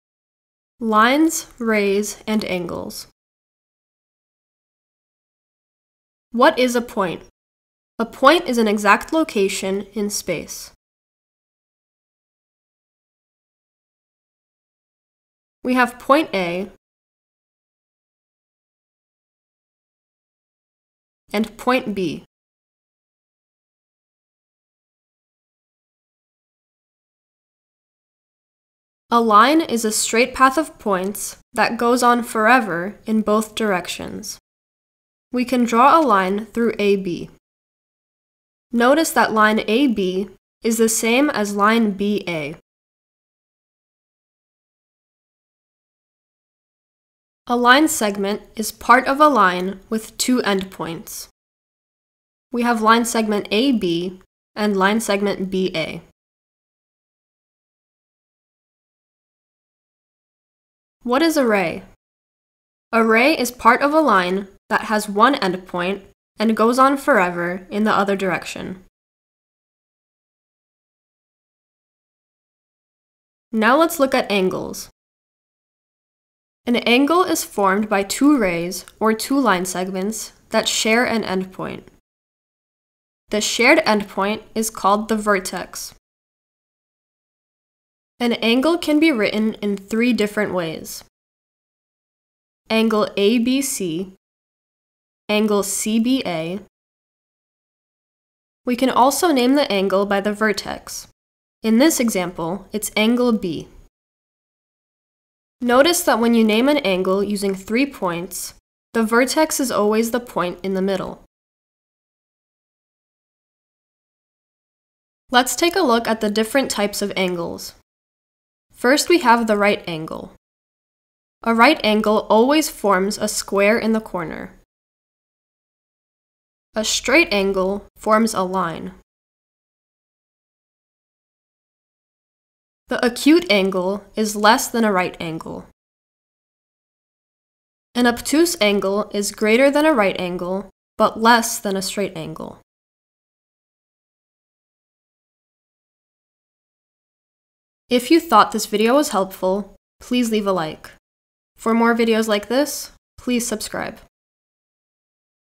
Lines, Rays, and Angles. Grade 4.
This video is designed to teach students how to identify and draw points, lines, line
segments, rays, and angles. Grade 4. Lesson 10.1.